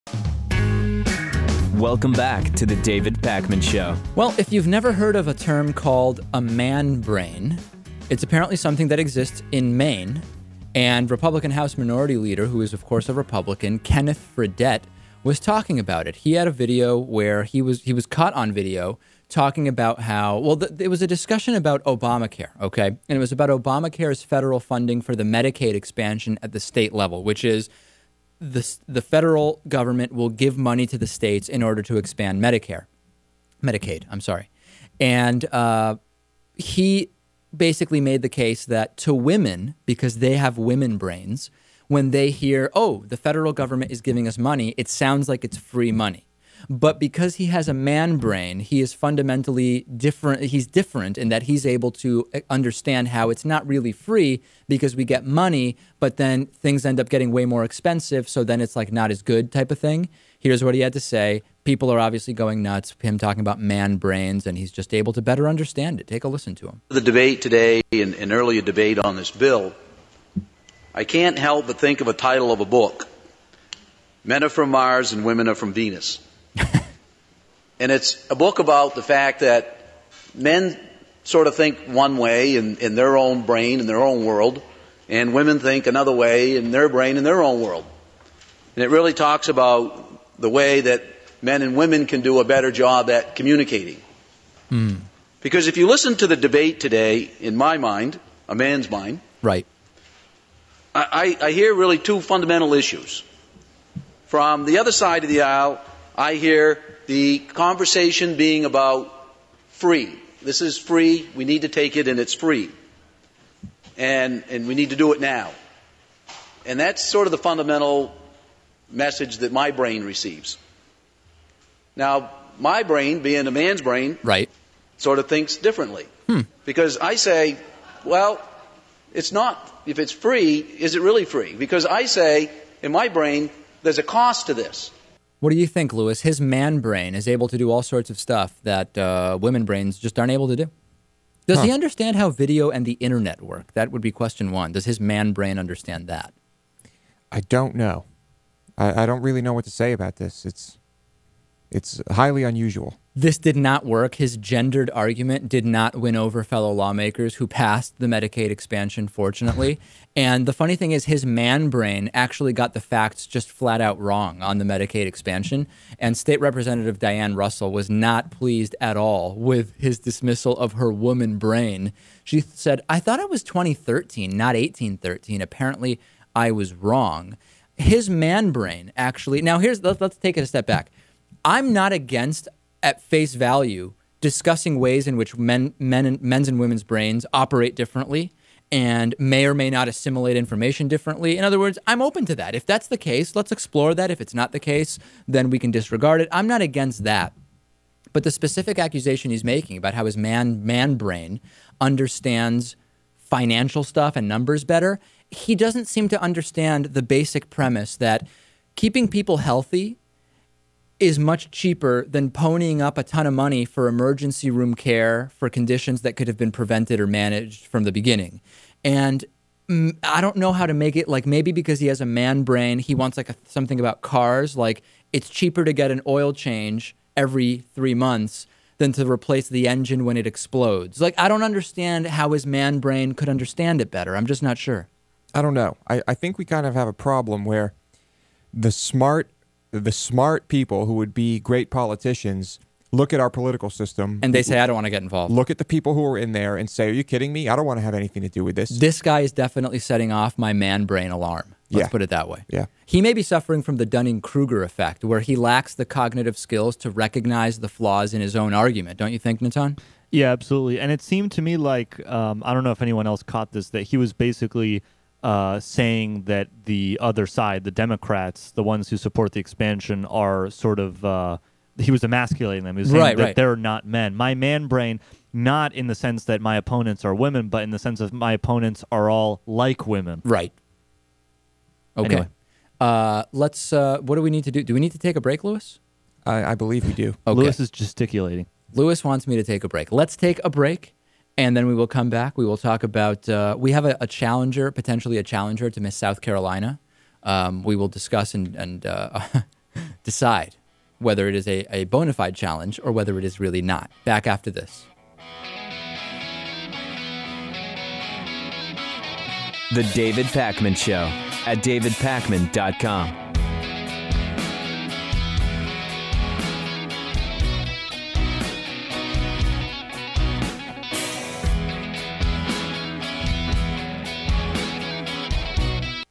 Republican's "Man Brain" Makes Him More Rational About Money
--Maine Republican House Minority Leader Kenneth Fredette says that his "man brain" makes him more rational about money.

--On the Bonus Show: Editing Louis' camping video, Spielberg predicts changes in film industry, new eye layer, more...

24/7 Voicemail Line: (219)-2DAVIDP